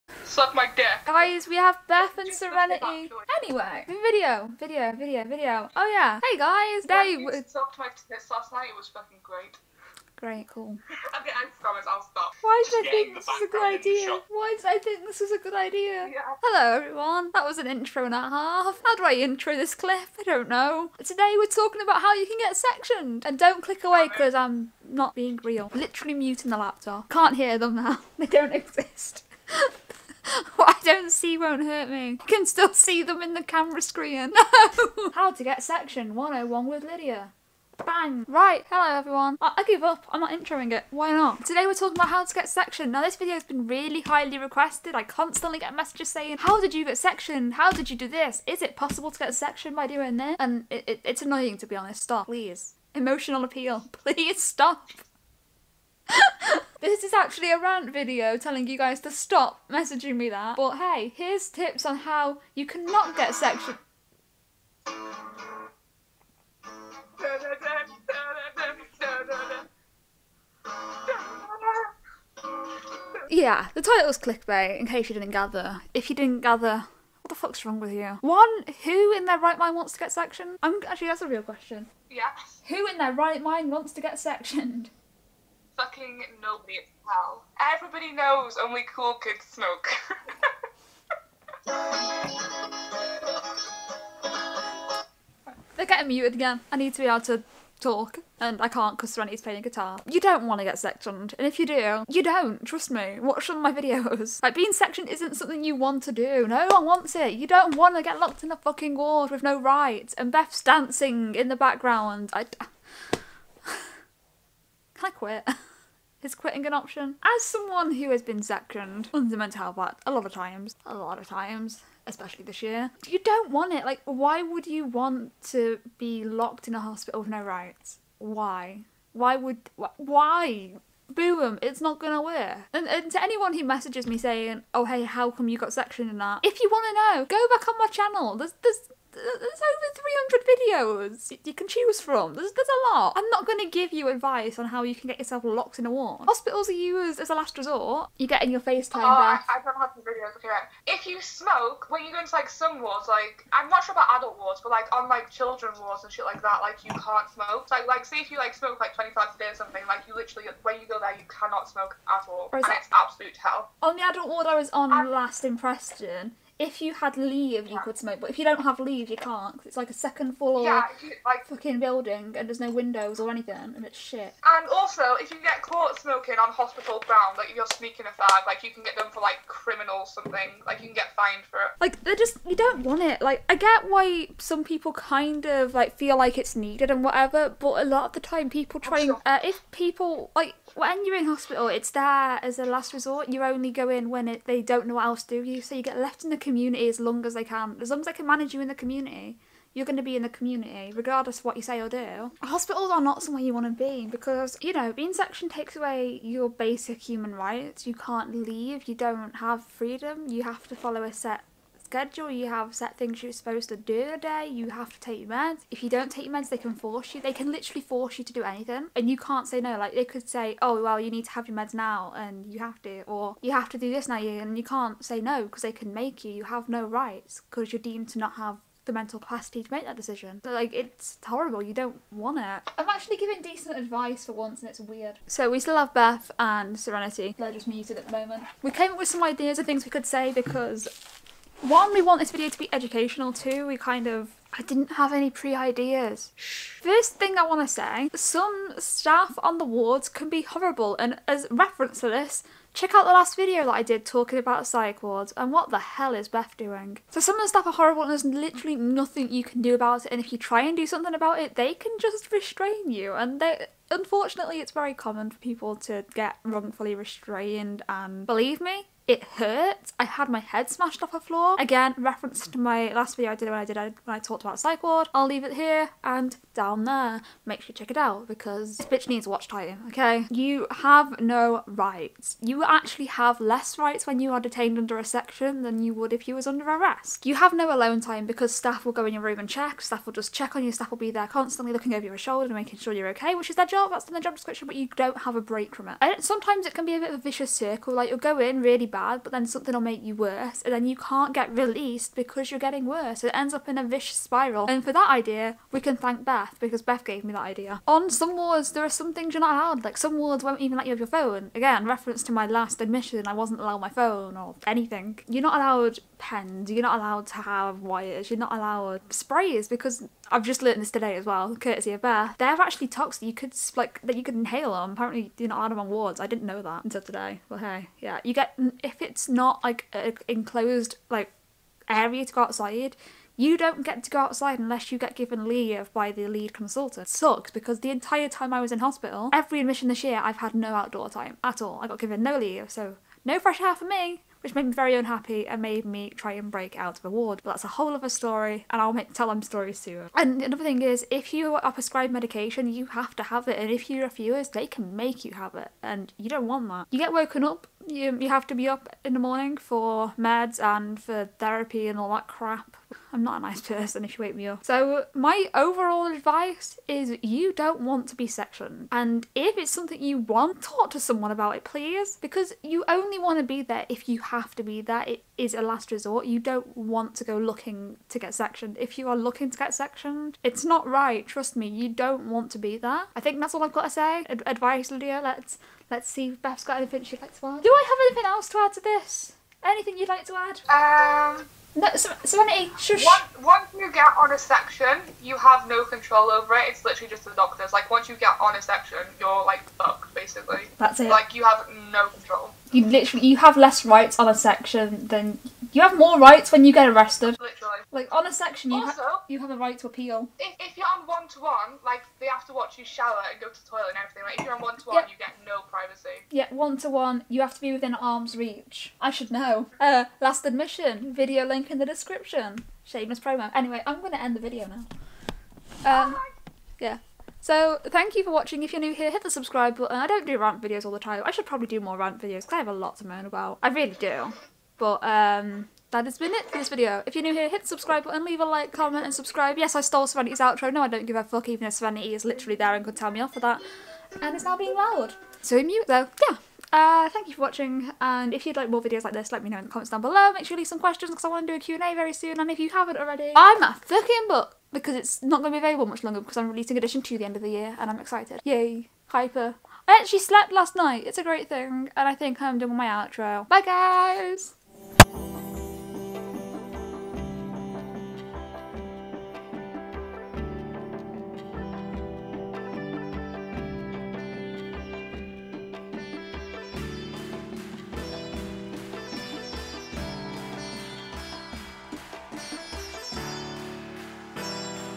So you want to be sectioned?
this is possibly one of my favourite rant videos I've done, I directly address comments in which I've been asked "how can I be sectioned" or "will this get me sectioned"

I stopped uploading rant videos ages ago but I'm at a point with comments like these that I feel like I need to address it.

Support Me:
(don't feel like you have to do this I'm putting it here because I don't have a ton of money to put into youtube videos so this is what I've decided to try)

My Equipment

NEW LENS
New Vlogging Camera
New Vlogging Camera Lens